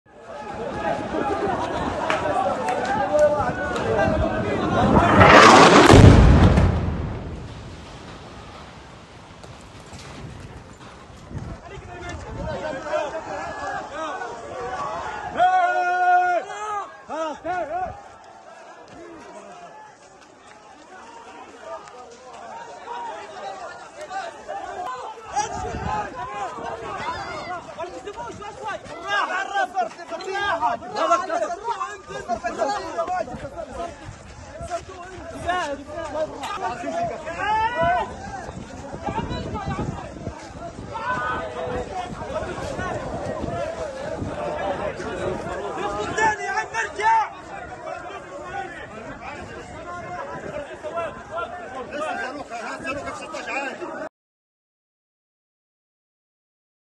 غارة جوية إسرائيلية تدمر برجاً سكنياً في وسط غزة | AFP
تظهر لقطات مصوّرة لحظة تعرّض برج المغاري السكني في مخيم البريج في وسط قطاع غزة لغارة جوية إسرائيلية أدت إلى تدميره، إثر تحذير من الجيش الإسرائيلي بضرورة إخلائه.